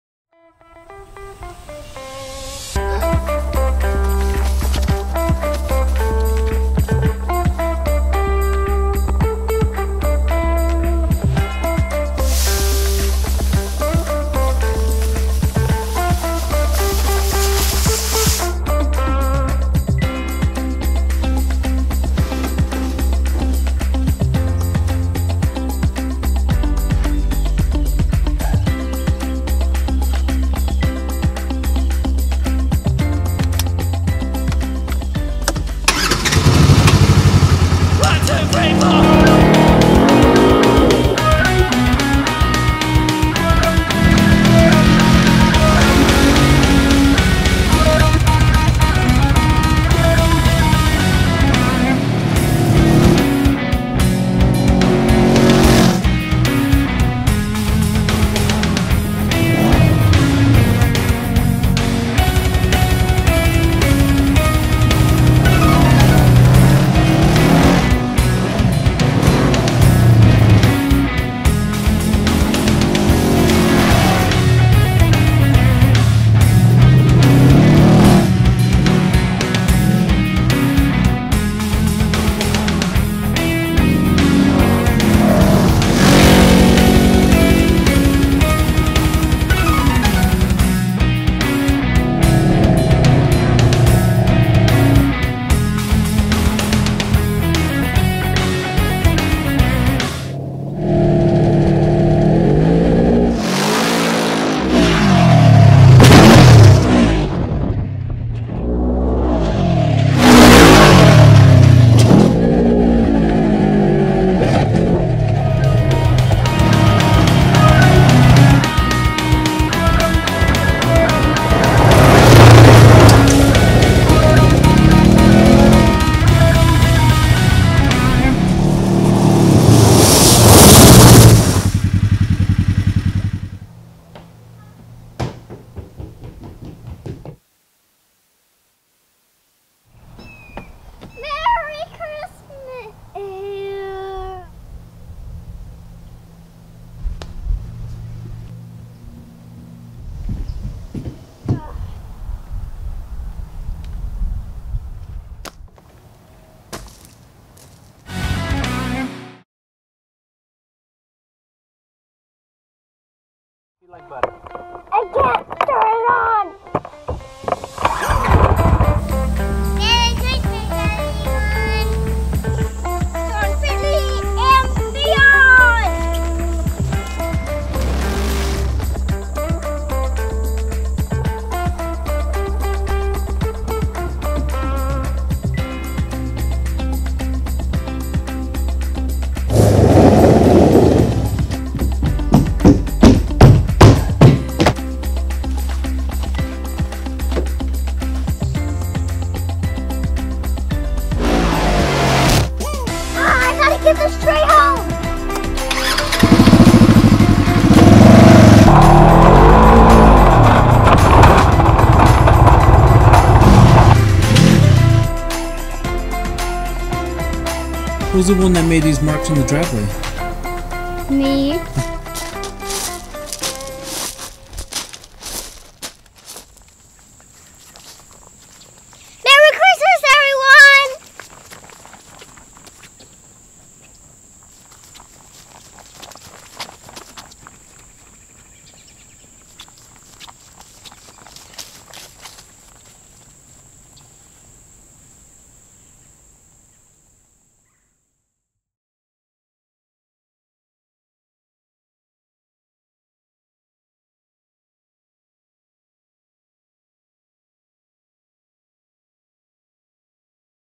A Leduc Christmas (kids)
6 year old Reed LeDuc brings you "A LeDuc Christmas" ( kids version) with his awesome Utv and his journey to bring home a Christmas Tree, but watch til the end and see how it goes for this kid! Co-starring is his 3 year old sister Addie LeDuc as she has her hopes up for a perfect tree! Share our fun video with your friends and family. Filmed at our home with mom and dad.